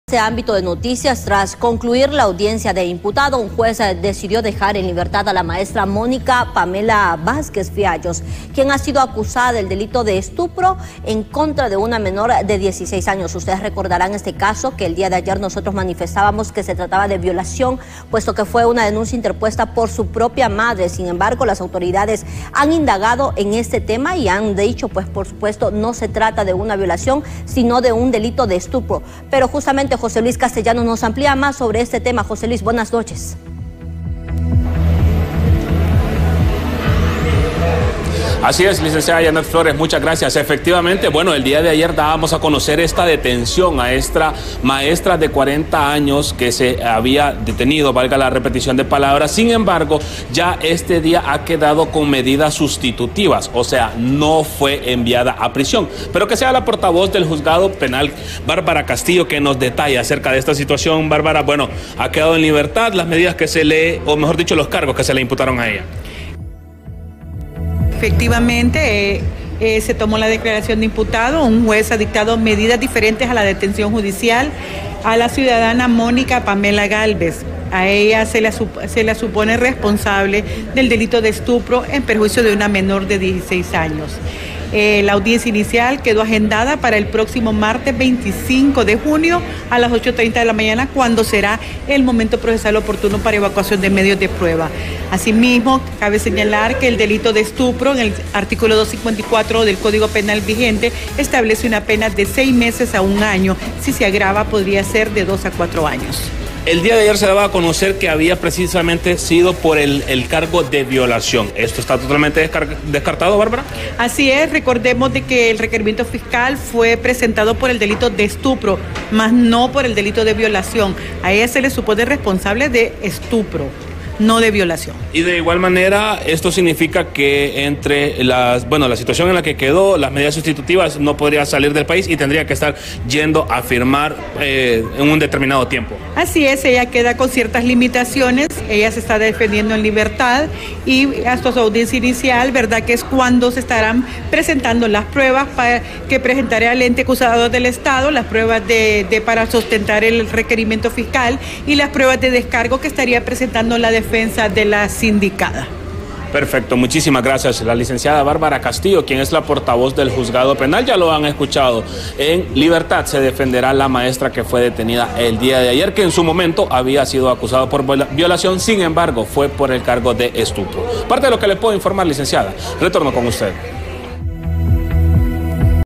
Maestra acusada de abuso sexual es puesta en libertad
El delito que se le imputa es de estupro.